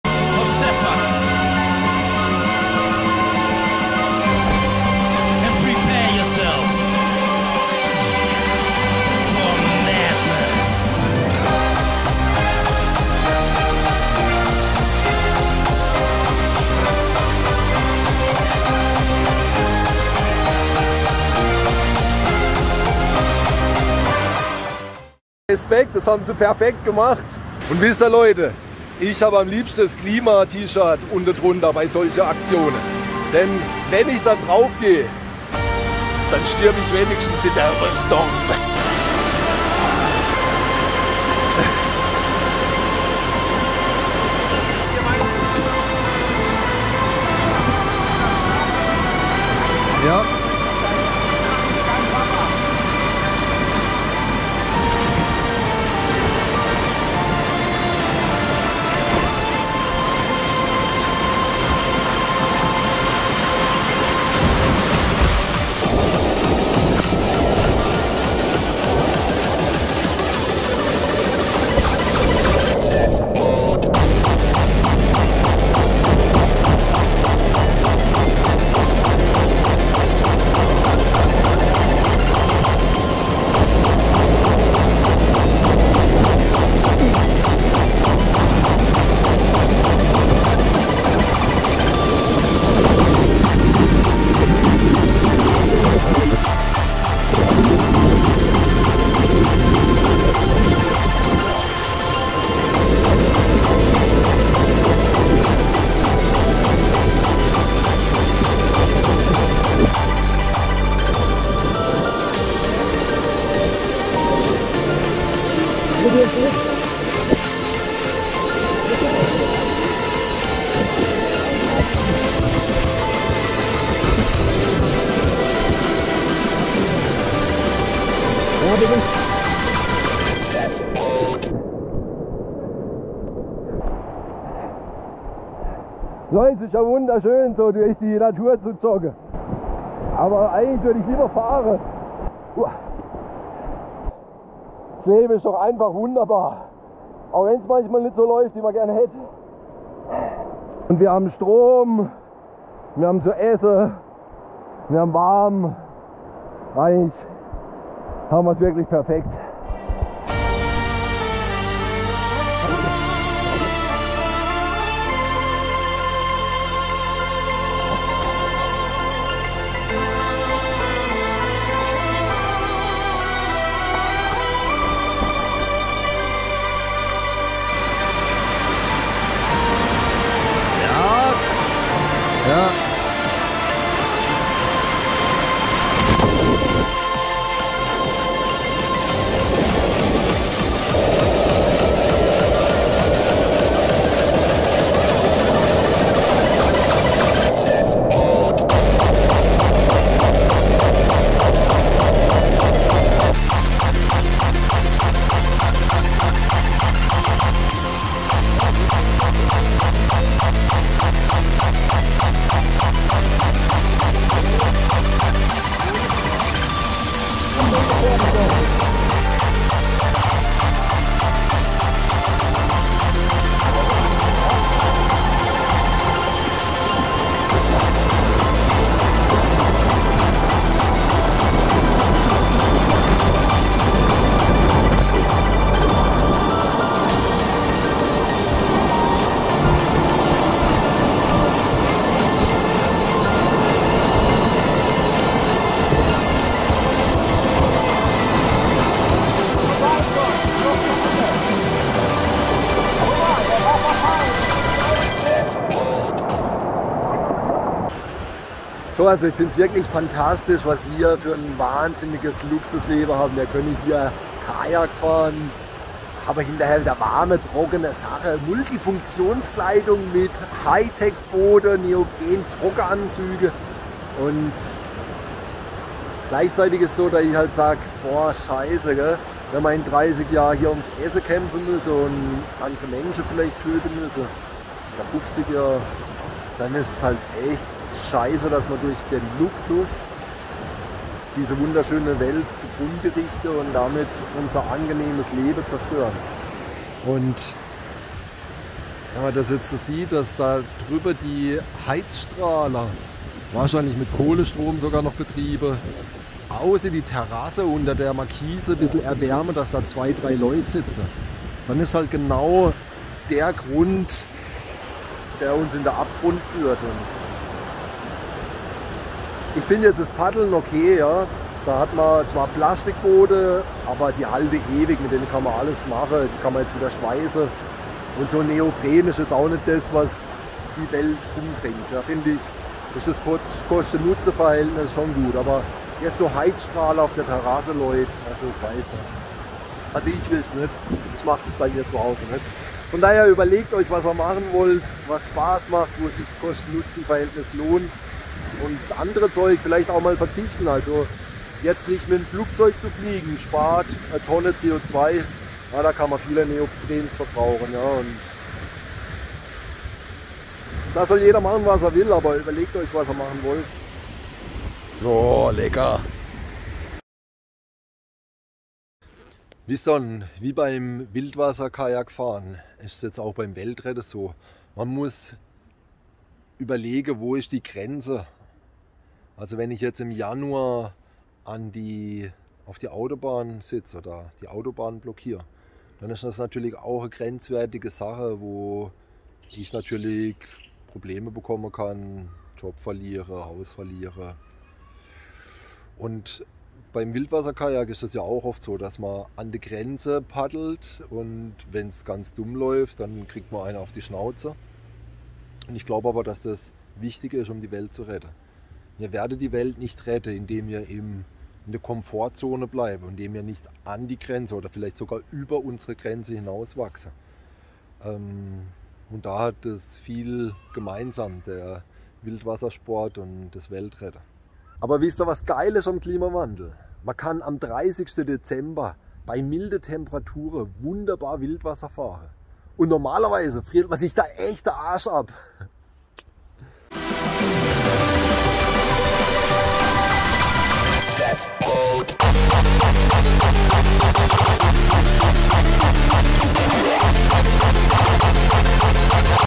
Paddeln for Future!°
Weltretten und Wildwasserkajakfahren haben etwas gemeinsam:
an die Grenzen gehen und über diese hinaus wachsen!
Die Grenze beim Kosten-Nutzen-Verhältnis ist oft schwierig zu ziehen.
Wilde Gutach 30.12.21